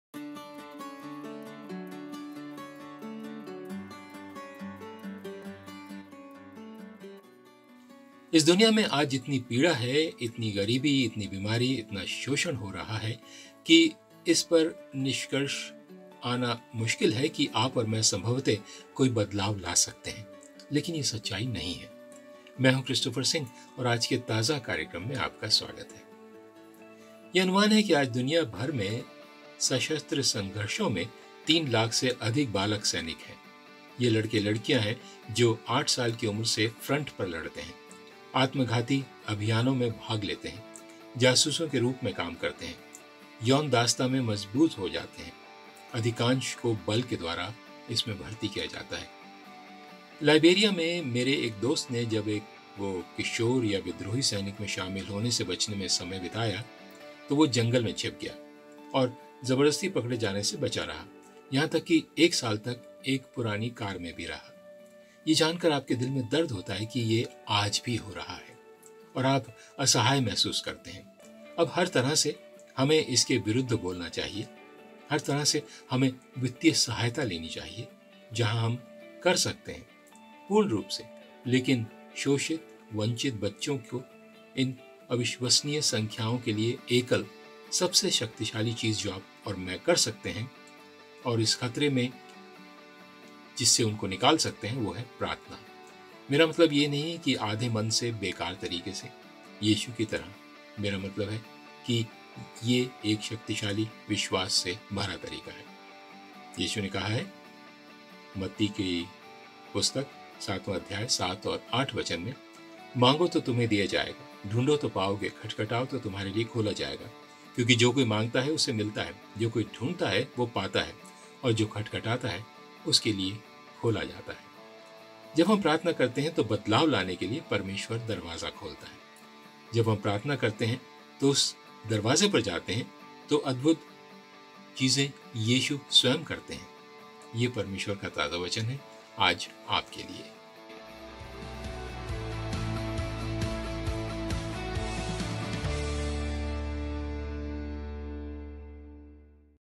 Fresh Thought for 2 January 2021
With so much suffering, so much poverty, so much sickness, so much exploitation going on in the world today, it’s easy to come to the conclusion that you and I can’t possibly make a difference. But nothing could be further from the truth.